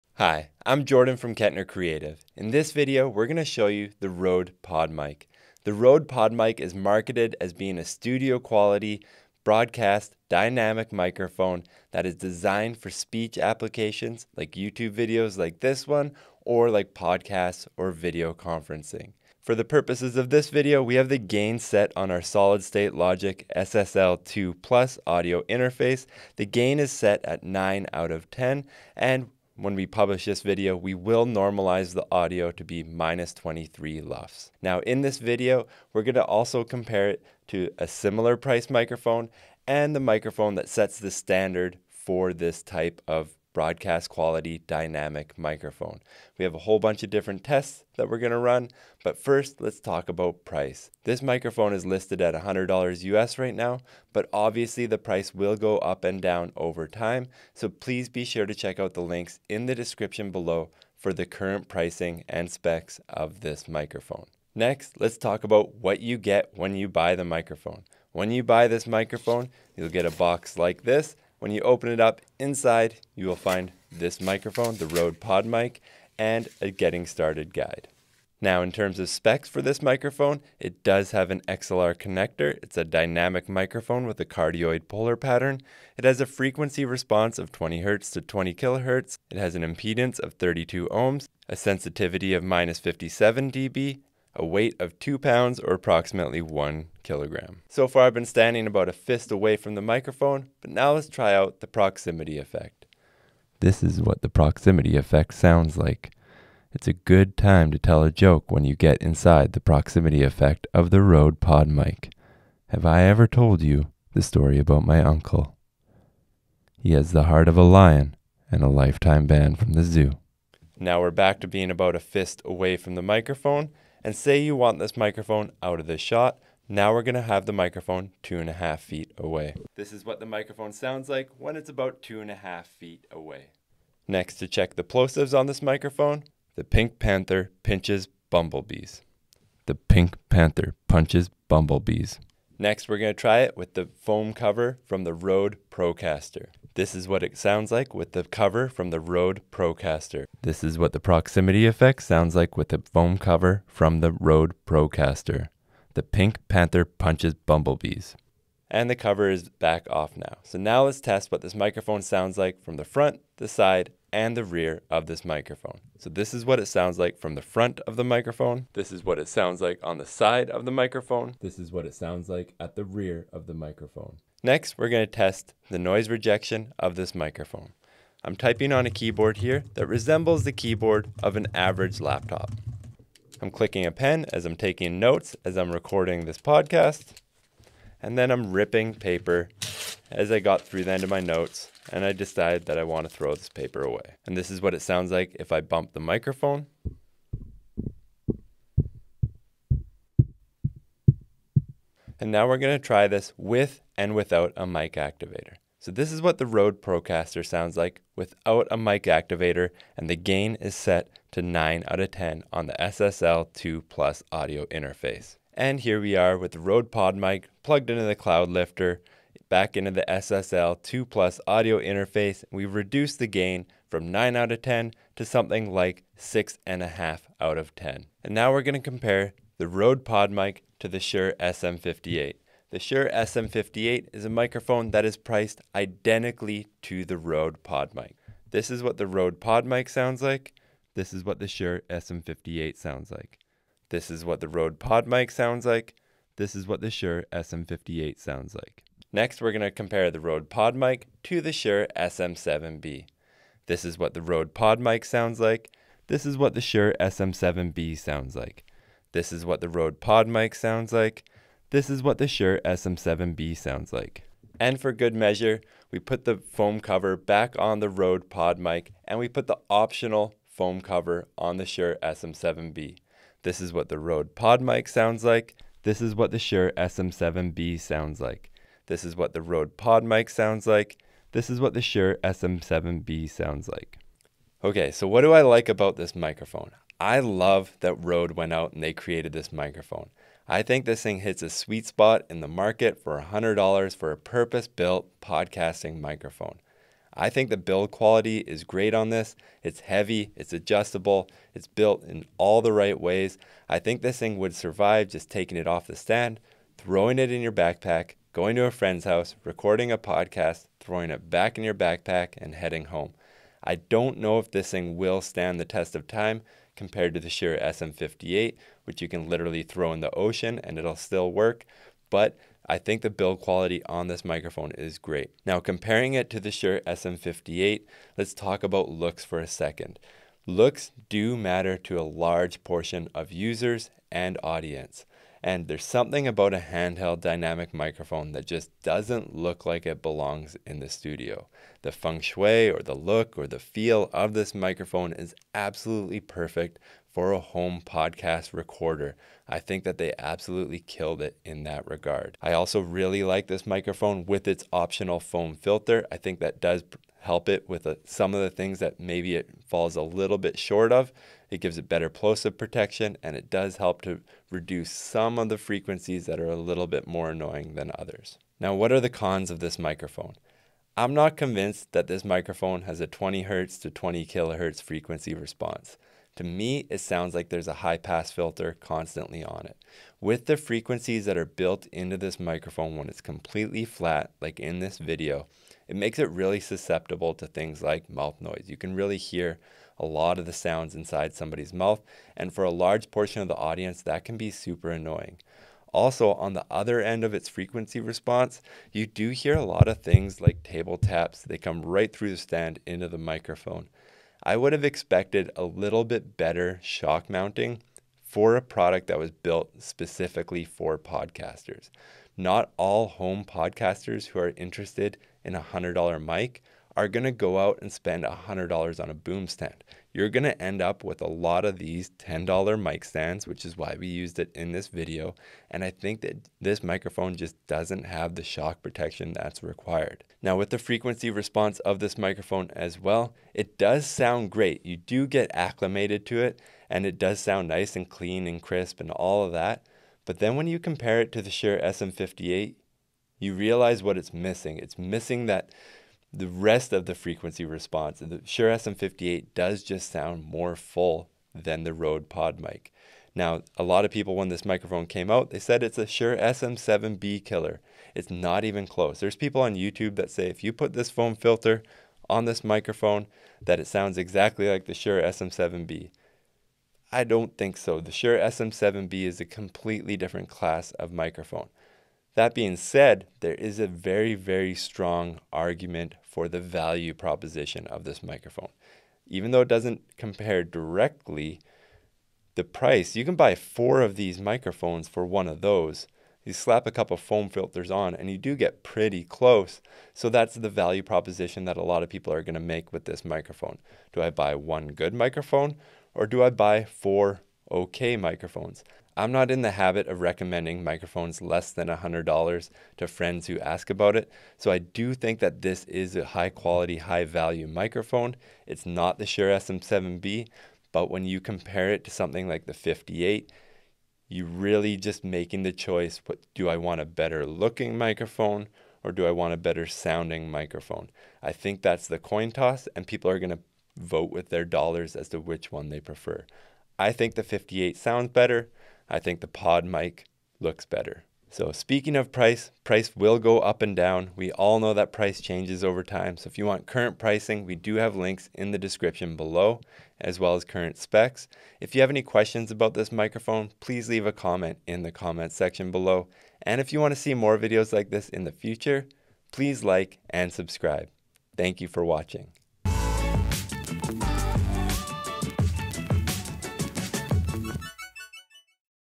Rode PodMic Dynamic Microphone | Review & Demo
In this video we review and demonstrate the Rode Podmic Dynamic Studio Microphone that was designed specifically for podcast recording.

CHAPTERS:
0:00 - Introduction
0:49 - Price
1:04 - What's In The Box?
1:18 - Specs
1:40 - Proximity Effect
2:01 - Distance Test
2:14 - Plosive Test
2:23 - Foam Windscreen
2:40 - Directional Response
2:54 - Background Noise Rejection
3:14 - Bump Test
3:22 - Mic Activator / Inline Preamp
3:48 - Rode Podmic vs Shure SM58
4:09 - Rode Podmic vs Shure SM7B
4:24 - Rode Podmic (windscreen) vs Shure SM7B (windscreen)
4:40 - Rode Podmic Pros
6:09 - Rode Podmic Cons
8:53 - Final Thoughts

SOFTWARE & TOOLS:
Final Cut Pro
MotionVFX Plugins
Affinity Photo